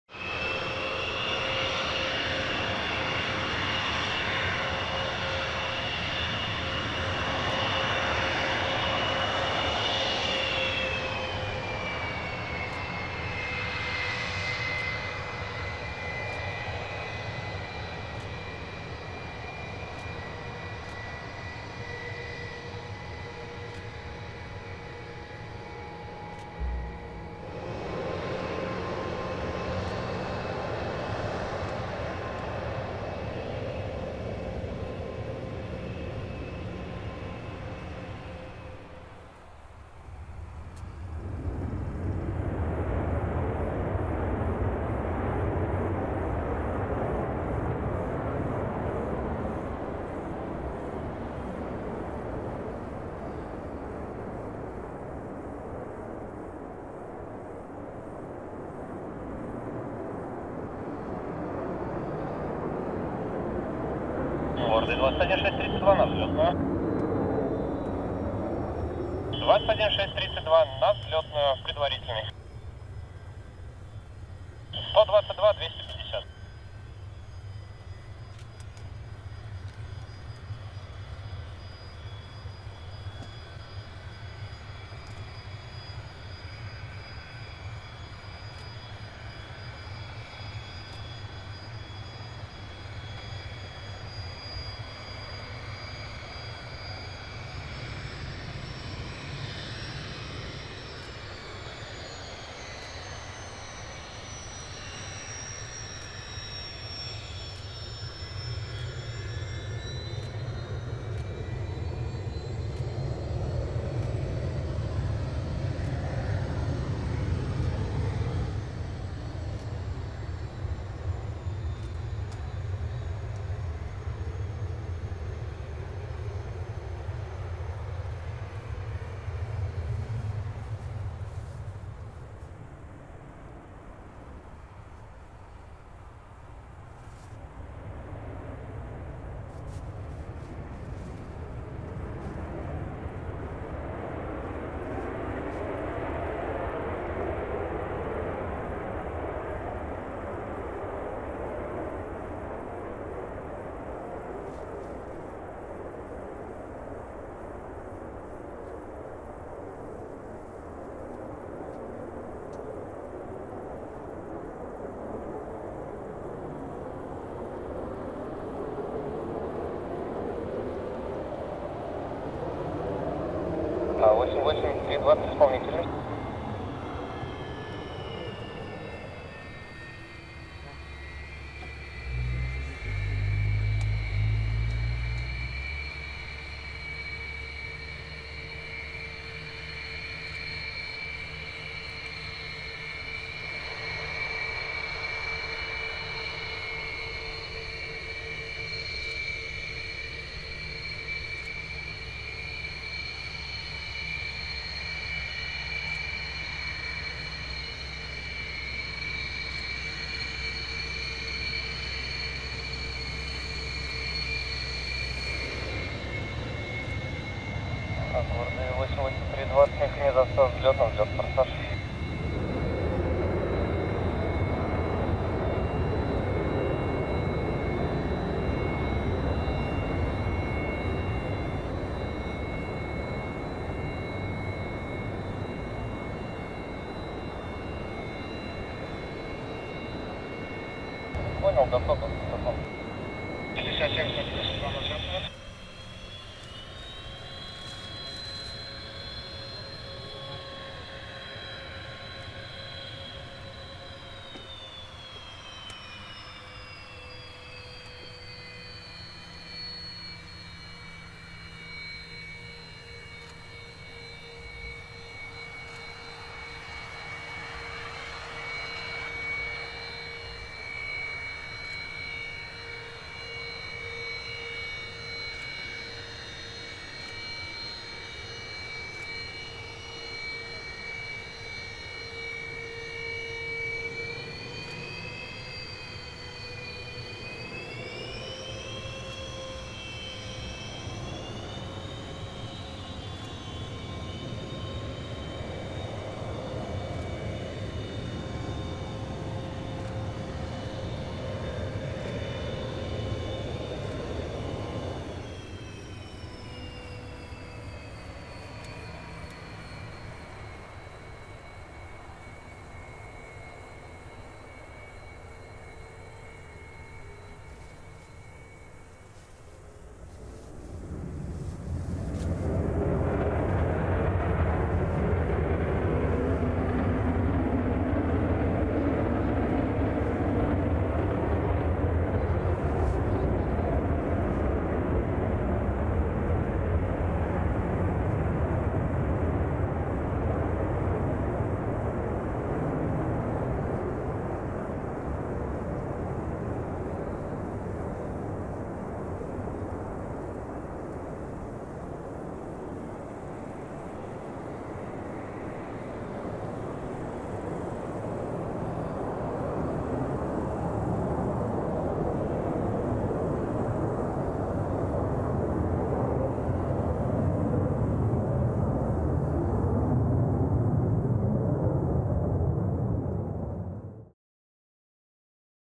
вылет Сухих: Су-35 Су-25 Су-34 Су-30
жаль марево сильное, Су-35 без форсажа и переговоры не все записались